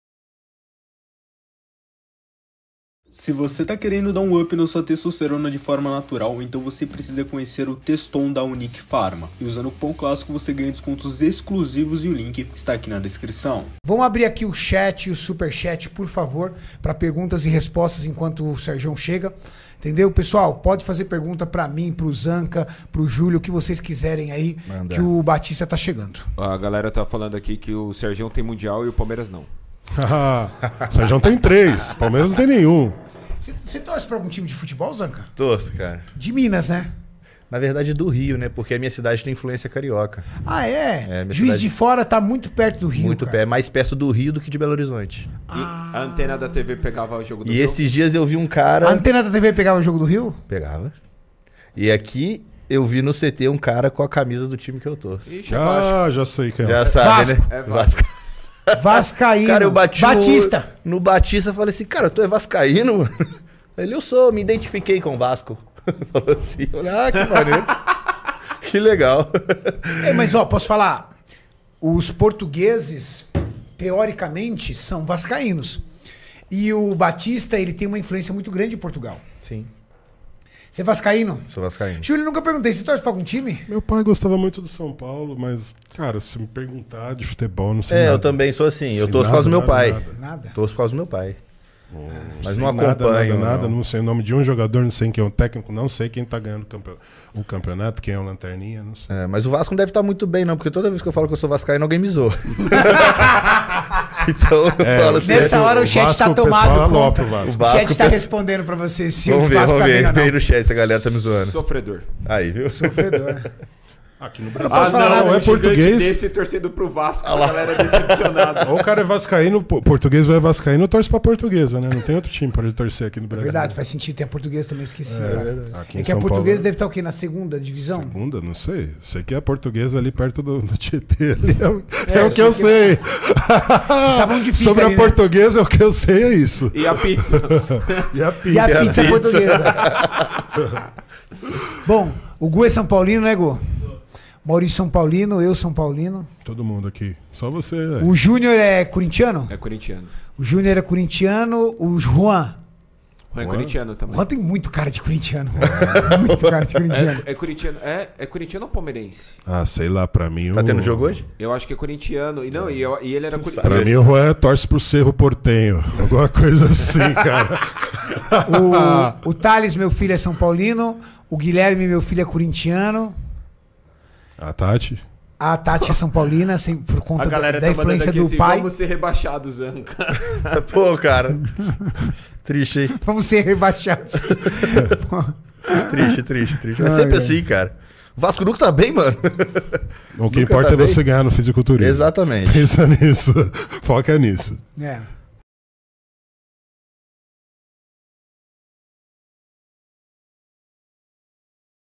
GABRIEL ZANCANELLI REVELA SEU TIME DE CORAÇÃO E É ZOADO POR BALESTRIN
⚡ MESTRE(S) DO VÍDEO:
@Zancanelli E @balestrinchannel8459
💰GANHE 3 MIL R$ SEM SAIR DE CASA💰